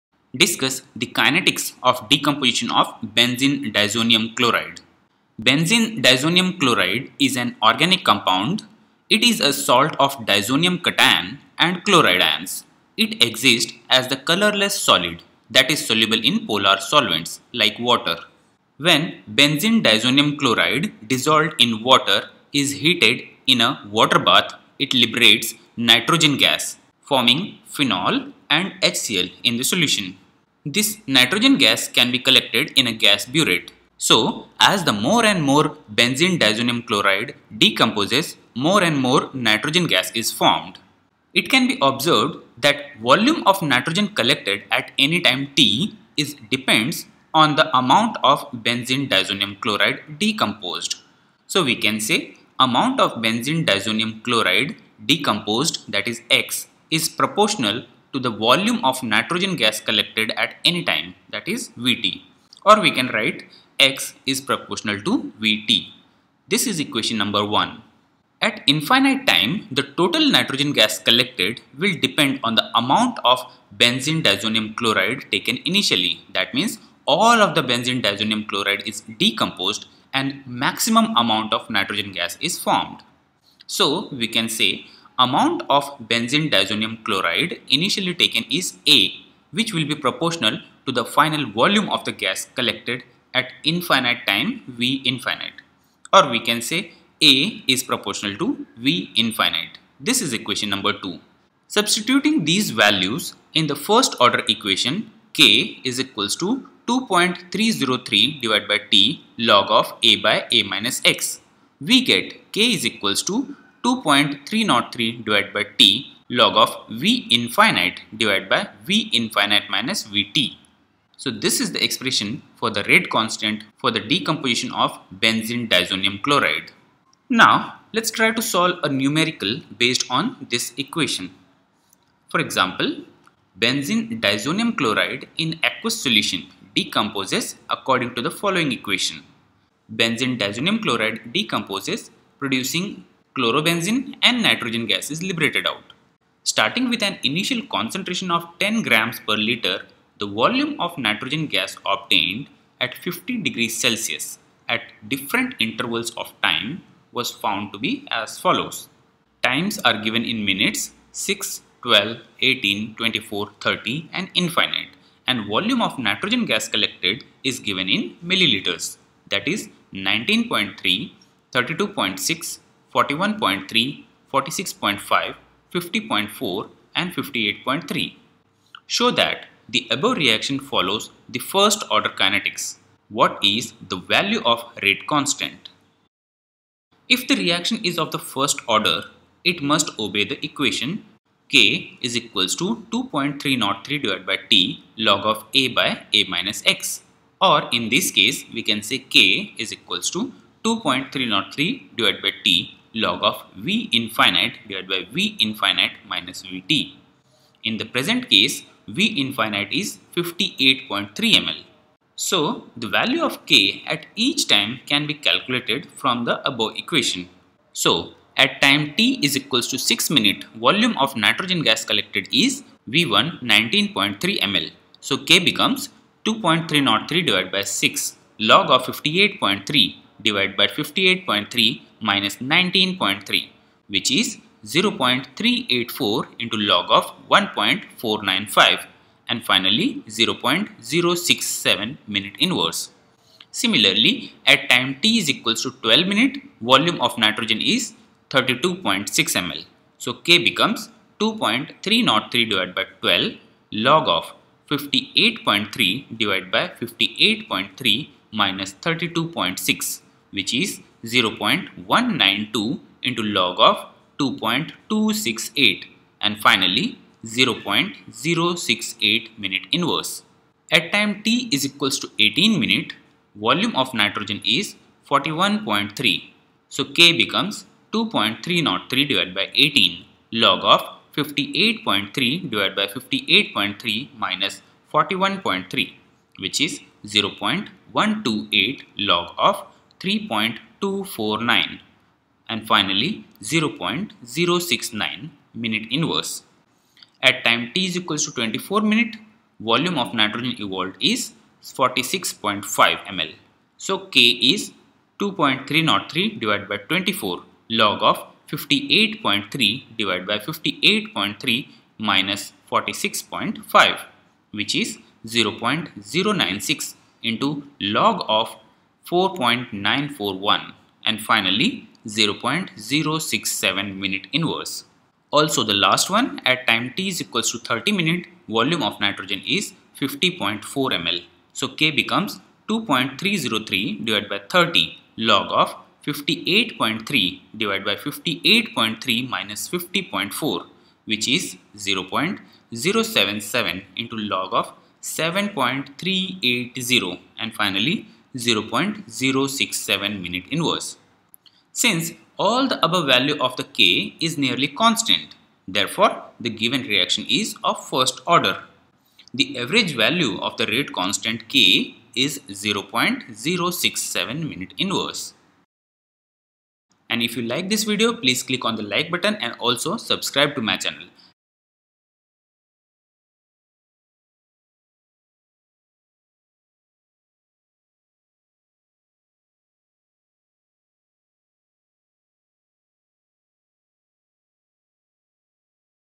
kinetics of Decomposition of Benzene Diazonium Chloride | Chemical Kinetics | Physical Chem
Benzenediazonium chloride is an organic compound with the formula [C6H5N2]Cl. It is a salt of a diazonium cation and chloride. It exists as a colorless solid that is soluble in polar solvents including water. It is the parent member of the aryldiazonium compounds, which are widely used in organic chemistry. Because the salt is unstable, it is not commercially available but is prepared upon demand.

C_6H_5N=NCl + H_2O \longrightarrow C_6H_5OH + N_2 + HCl

It can be observed that, volume of { N }_{ 2 } collected at time t is depends on the amount of { C }_{ 6 }{ H }_{ 5 }N=NCl decomposed.  left( { Amount of { C }_{ 6 }{ H }_{ 5 }N=NCl decomposed (x) } right)  propto  left( { Volume of { N }_{ 2 } gas collected at any time left( { V }_{ t } right)  } right)   i.e. x propto  { V }_{ t }   ...(1)  At infinite time the total { N }_{ 2 } gas collected will depend on the amount of { C }_{ 6 }{ H }_{ 5 }N=NCl taken initially.  left( { Amount of { C }_{ 6 }{ H }_{ 5 }N=NCl initially taken (a) } right)  propto  left( { Volume of { N }_{ 2 } gas collected at infinite time left( { V }_{ infty  } right)  } right)   i.e. a propto  { V }_{ infty  }   ...(2)  Substituting these values in the first order equation. k = frac { 2.303 }{ t } log { frac { a }{ a-x }  }   we get,  boxed { k = frac { 2.303 }{ t } log { frac { { V }_{ infty  } }{ { V }_{ infty  }-{ V }_{ t } }  }  }

Numerical: Benzene diazonium chloride in aqueous solution decomposes according to the equation  C_{ 6 }H_{ 5 }N=NCl longrightarrow  C_{ 6 }H_{ 5 }Cl + N_{ 2 }  Starting with an initial concentration of 10g/litre, the volume of N_{ 2 } gas obtained at 50°C at different intervals of time was found to be as follows:                           Time (minutes):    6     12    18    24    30     infty       Vol. of { N }_{ 2 } (ml):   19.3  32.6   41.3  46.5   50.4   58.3    Show that the above reaction follows the first order kinetics. What is the value of the rate constant?  Solution: If the reaction is of the first order, it must obey the equation,  k = frac { 2.303 }{ t } log { frac { a }{ a-x }  }   or boxed { k = frac { 2.303 }{ t } log { frac { { V }_{ infty  } }{ { V }_{ infty  }-{ V }_{ t } }  }  }   In the present case, { V }_{ infty  } = 58.3ml  The value of k at each time can be calculated as follows:  1) at time t = 6 min volume of { N }_{ 2 } evolved { V }_{ t } = 19.3  k = frac { 2.303 }{ 6 } log { frac { 58.3 }{ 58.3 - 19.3 }  }  = 0.384times log { left( 1.495 right)  }  = 0.067{ min }^{ -1 }  2) at time t = 12 min volume of { N }_{ 2 } evolved { V }_{ t } = 32.6  k = frac { 2.303 }{ 12 } log { frac { 58.3 }{ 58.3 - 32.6 }  }  = 0.192times log { left( 2.268 right)  }  = 0.068{ min }^{ -1 }  3) at time t = 18 min volume of { N }_{ 2 } evolved { V }_{ t } = 41.3  k = frac { 2.303 }{ 18 } log { frac { 58.3 }{ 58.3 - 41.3 }  }  = 0.128times log { left( 3.429 right)  }  = 0.069{ min }^{ -1 }  4) at time t = 24 min volume of { N }_{ 2 } evolved { V }_{ t } = 46.5  k = frac { 2.303 }{ 24 } log { frac { 58.3 }{ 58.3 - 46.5 }  }  = 0.096times log { left( 4.941 right)  }  = 0.067{ min }^{ -1 }  5) at time t = 30 min volume of { N }_{ 2 } evolved { V }_{ t } = 50.4  k = frac { 2.303 }{ 30 } log { frac { 58.3 }{ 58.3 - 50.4 }  }  = 0.077times log { left( 7.380 right)  }  = 0.067{ min }^{ -1 }  Since the value of k is nearly constant, given reaction is of the first order. The average value of the rate constant = 0.067{ min }^{ -1 }